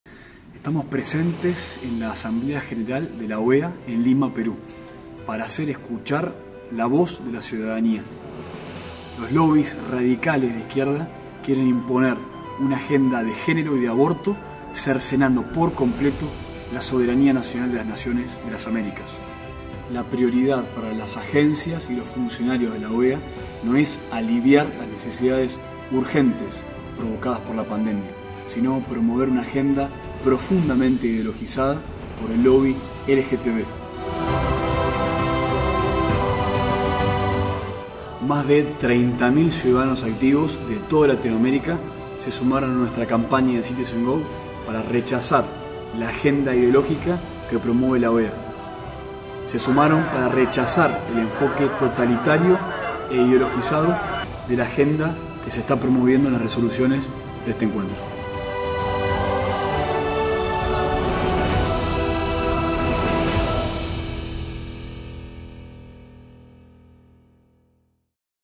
OEA: a la mujer la define la biología, NO la ideología
Desde CitizenGO estuvimos presentes en la Asamblea General de la Organización de Estados Americanos en Lima, Perú para entregar más de 30 mil firmas de ciudadanos activos que exigen a sus representantes ante la OEA que se rechace el enfoque idelógico y autoritario de las resoluciones del encuentro. No estamos dispuestos a ceder la soberanía nacional de nuestros países y que lobbies totalitarios decidan por nosotros.